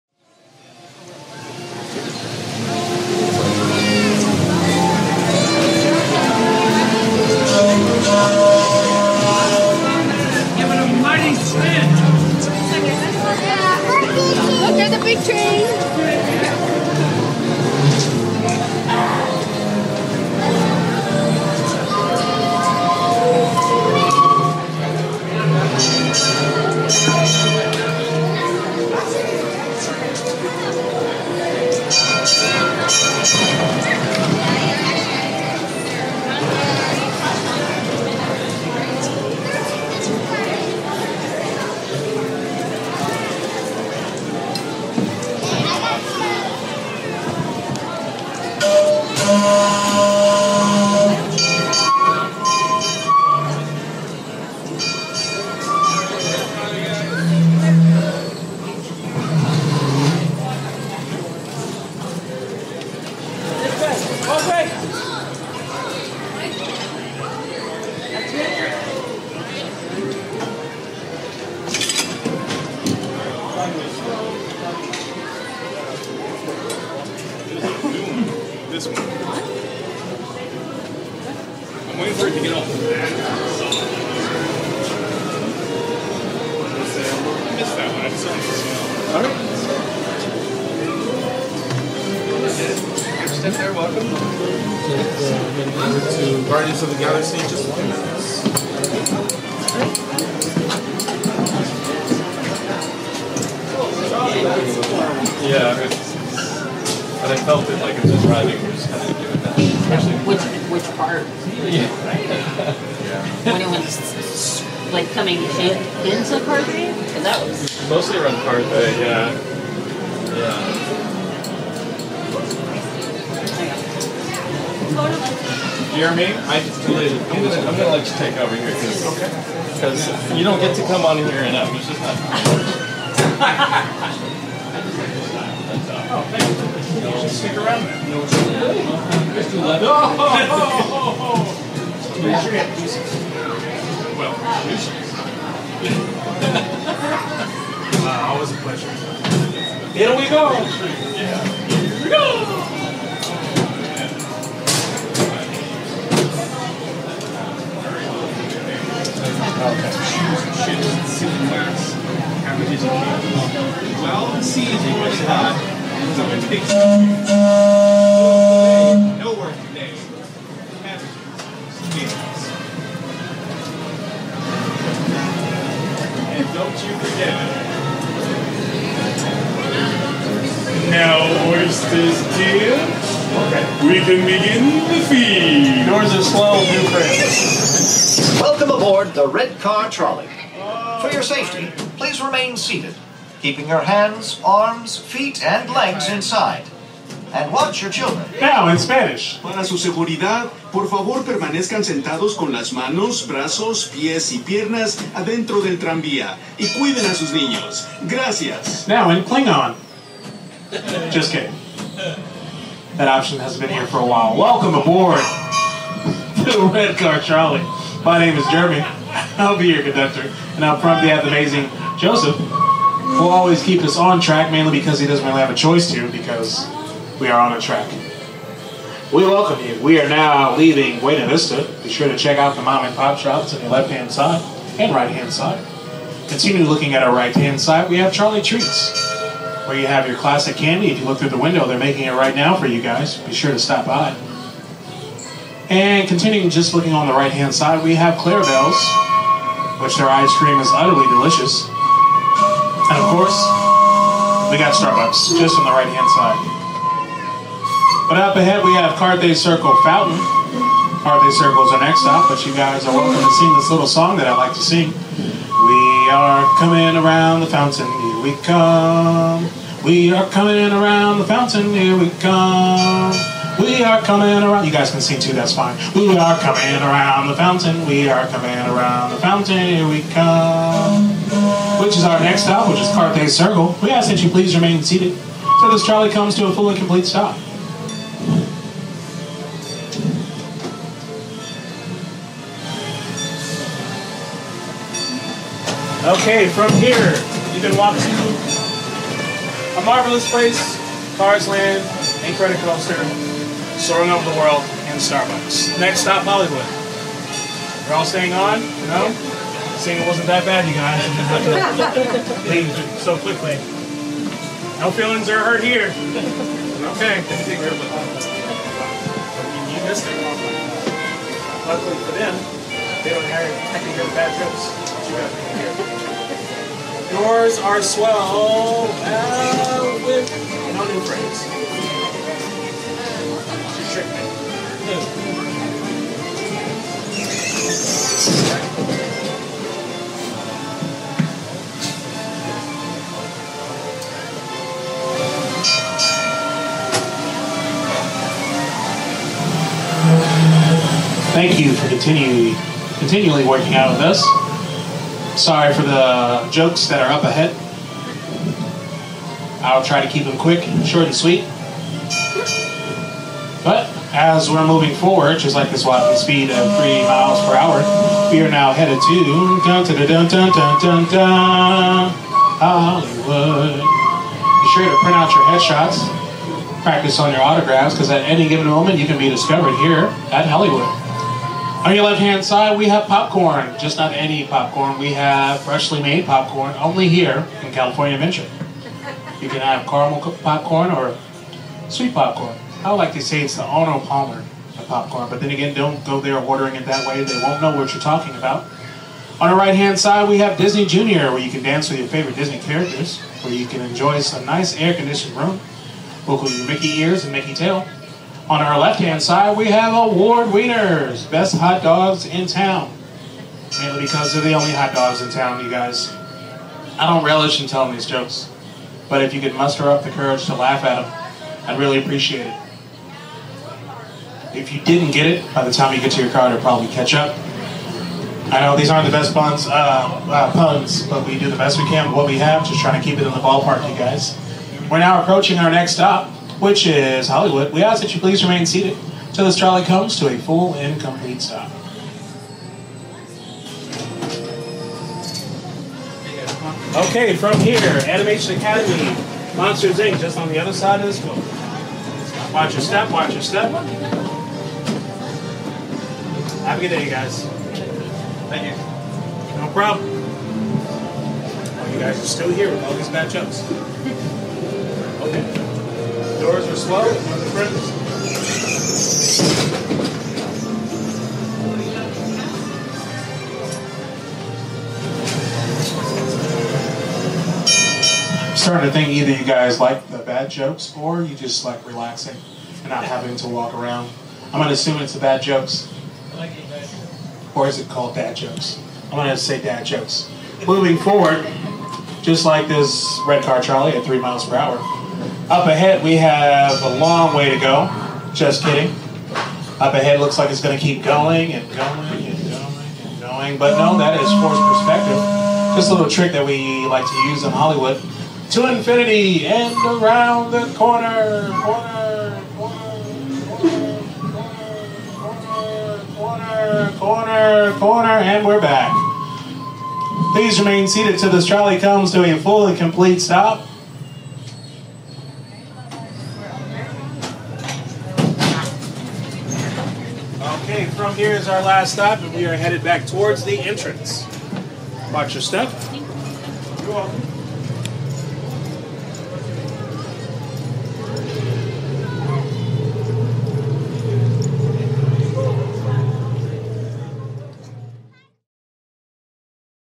Red Car Trolley
Disneyland - Red Car Trolley

Ultimately it’s a spiel full of puns.

Audio recorded on 1/16/23 around 12:00pm.